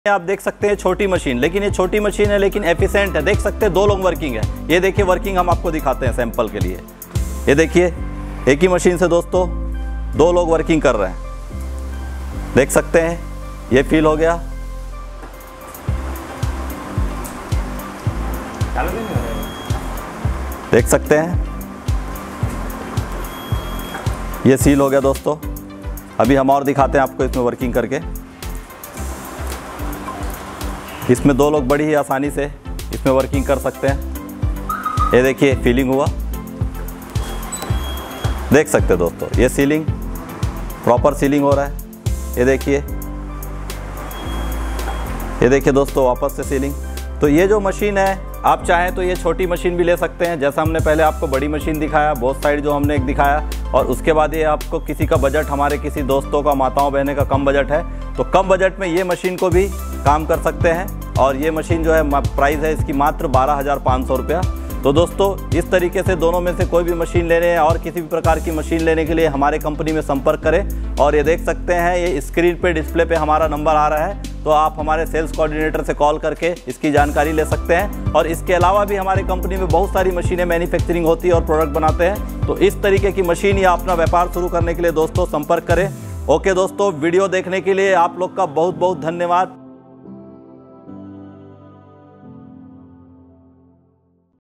Kickstart Your Business | Shree Enterprises Bhilai's Juice Packing and Sealing Machine
Kickstart Your Business with Shree Enterprises Bhilai's Juice Packing and Sealing Machine – Where Freshness Meets Efficiency

Are you ready to kickstart your business and ensure the highest quality for your products? Discover the power of Shree Enterprises Bhilai's juice packing and sealing machine – where freshness meets efficiency. In this video, we'll take you through the features and benefits of our state-of-the-art machine designed to streamline your packing process and keep your juices fresh for longer.

Shree Enterprises Bhilai offers a cutting-edge solution to enhance your production line, reduce downtime, and maximize productivity. Our juice packing and sealing machine is engineered for precision and reliability, making it an essential asset for any business looking to scale up and maintain the highest standards of product quality.
Shree Enterprises Bhilai, Juice Packing Machine, Sealing Machine, Business Kickstart, Freshness and Efficiency, Entrepreneurship, Business Growth,